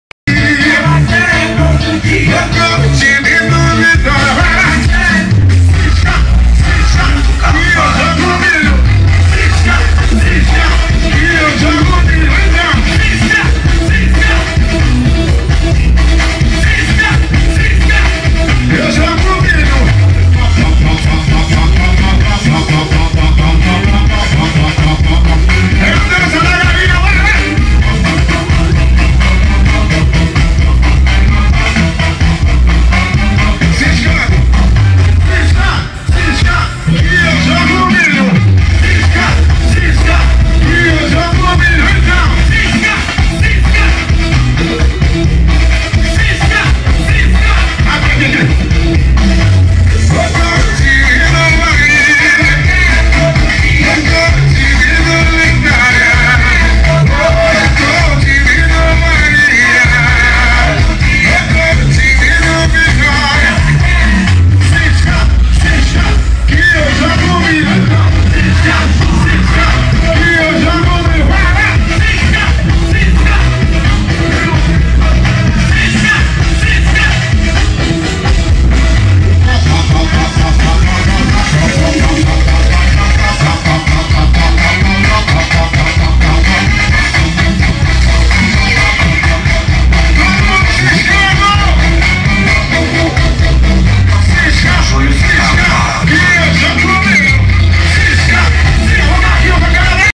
F-250 Prata basculante de Barreiras com a caçamba levantada fazendo o carnaval da galera no cais.mp4
Barreiras - BA - Carnaval 2012